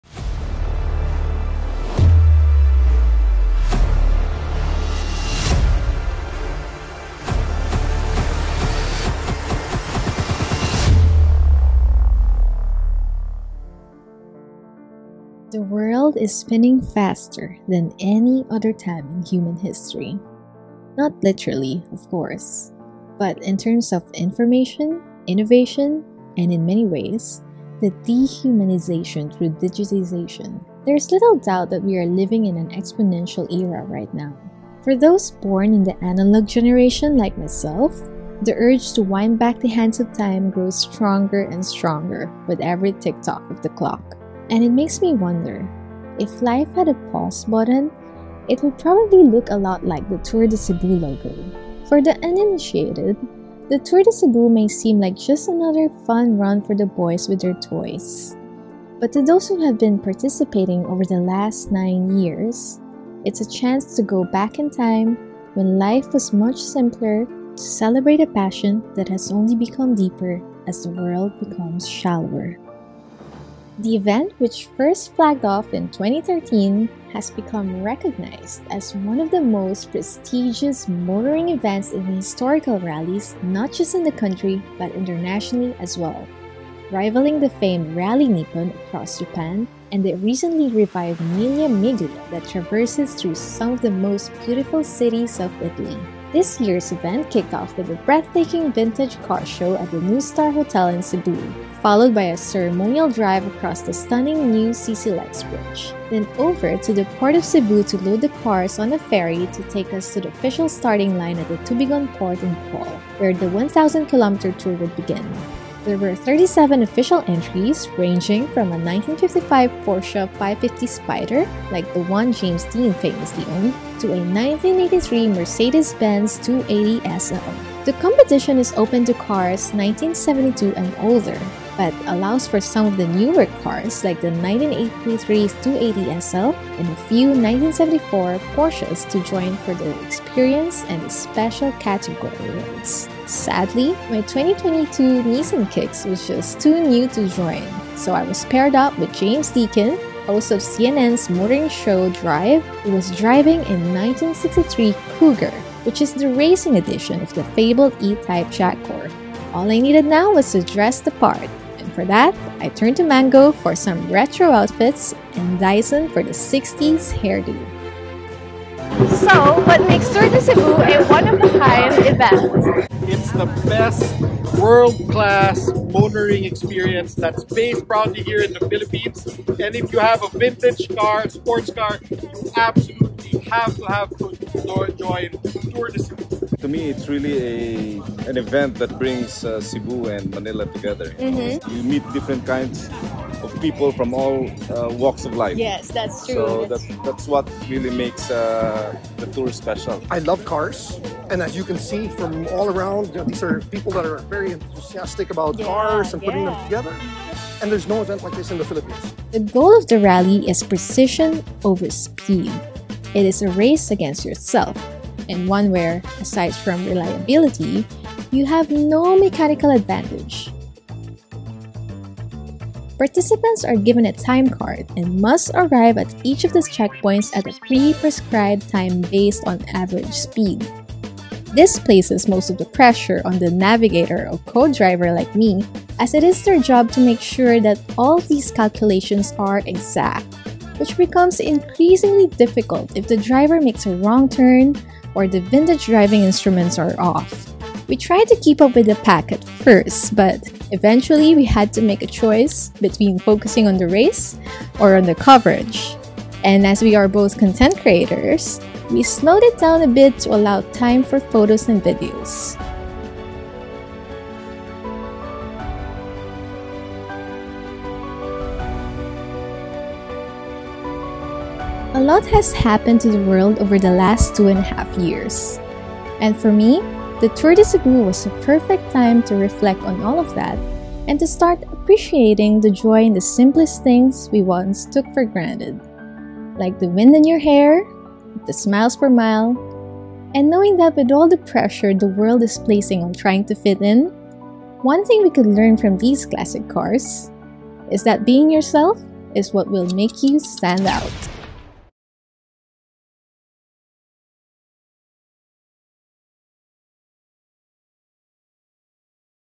🇵🇭🏁 Vintage Car Rally Tour De Cebu - Bohol Philippines 2022 🏎️
One more item on the Bucket list ticked!  Finally!

After years of just reading about it in magazines and websites, I was finally able to join this year’s Tour de Cebu—which is a one of a kind, 1000km historical rally across the island of Bohol. This is an absolute must for vintage car lovers. So if you’re ready to take a trip of a lifetime, jump in and I’ll show you what the event looks like through the windscreen of a 1963 Kougar.

Credits to:

@mr.odulio for the awesome drone shots
 @greek_maverick for my logo